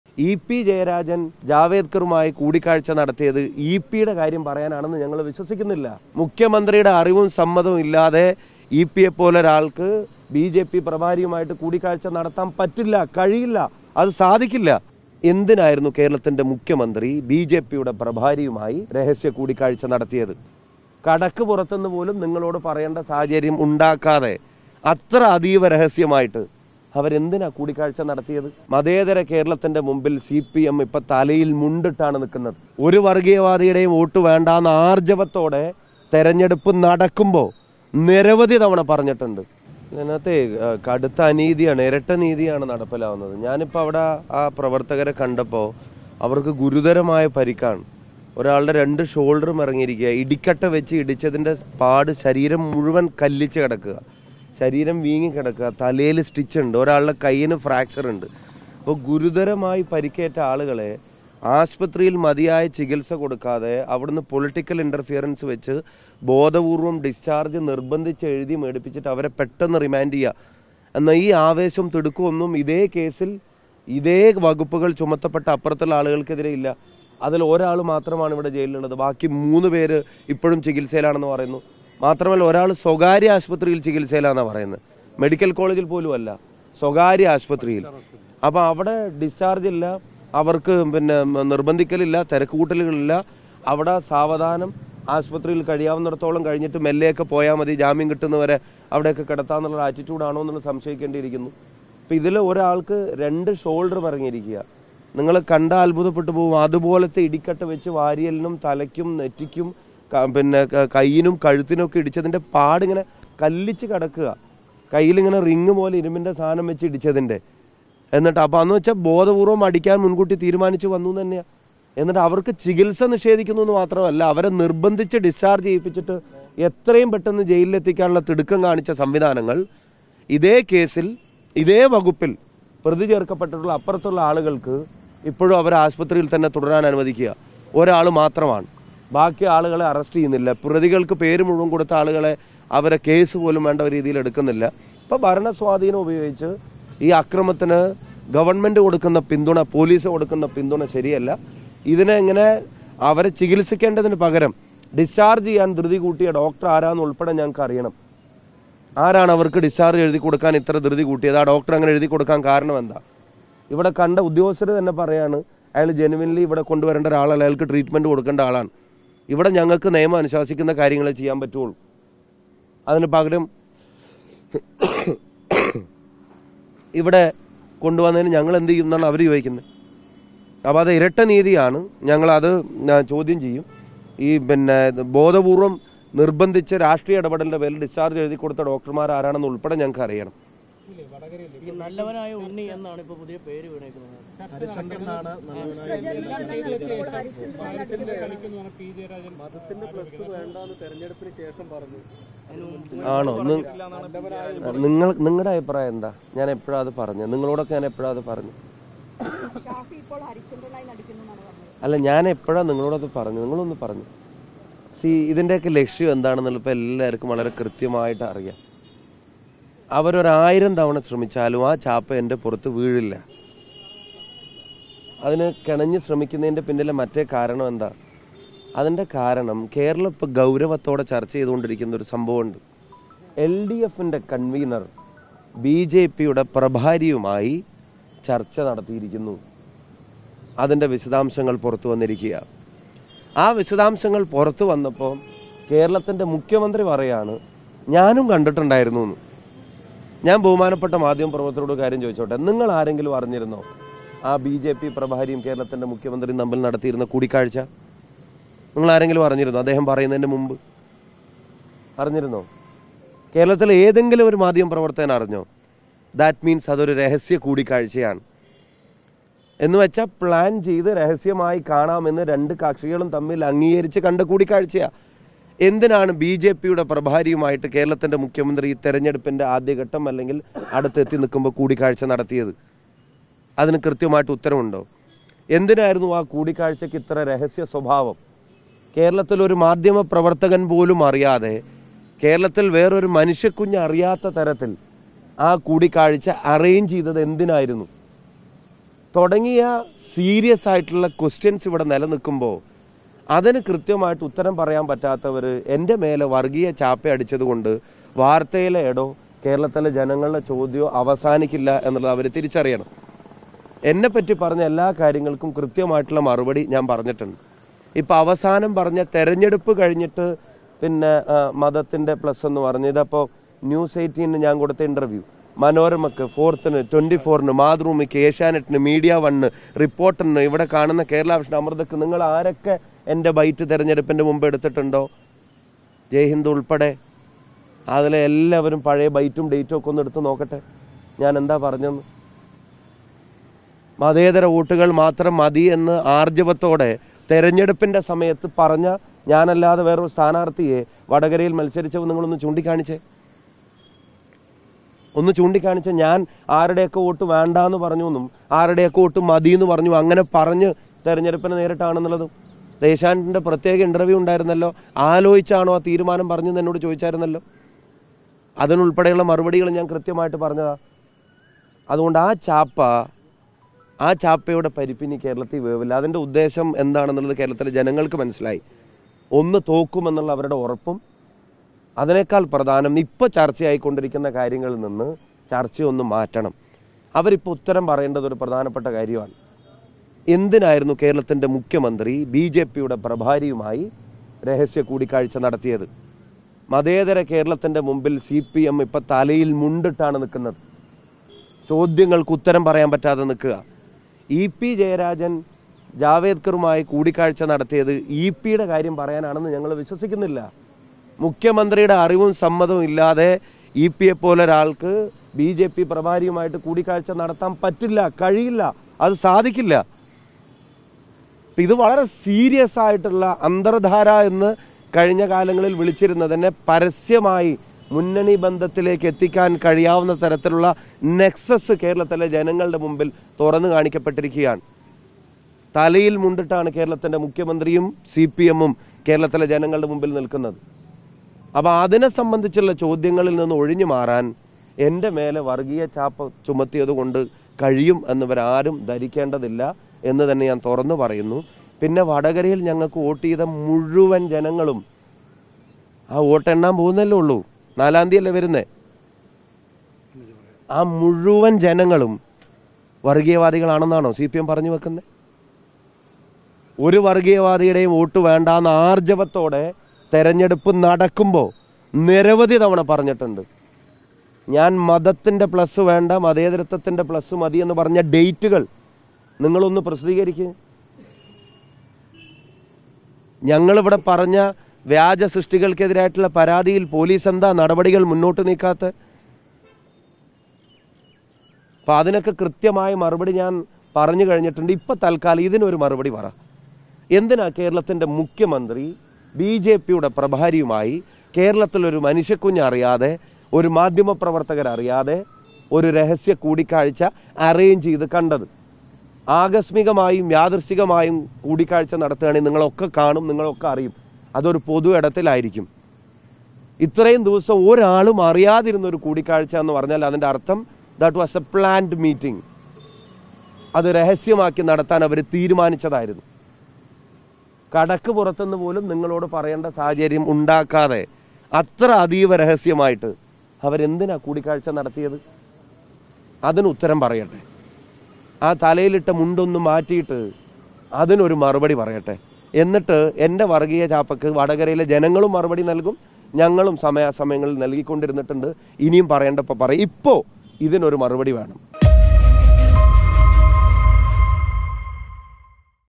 'ആരും അറിഞ്ഞില്ല മുഖ്യമന്ത്രി BJPയുടെ പ്രഭാരിയുമായി കൂടിക്കാഴ്ച നടത്തിയത്, അപ്പോൾ അത് രഹസ്യമല്ലേ...'
'ഒരു മാധ്യമ പ്രവർത്തകരും അറിഞ്ഞില്ല മുഖ്യമന്ത്രി BJPയുടെ പ്രഭാരിയുമായി കൂടിക്കാഴ്ച നടത്തിയത്...അപ്പോൾ അത് രഹസ്യമല്ലേ...' | Shafi Parambil

With Regards
Team RBC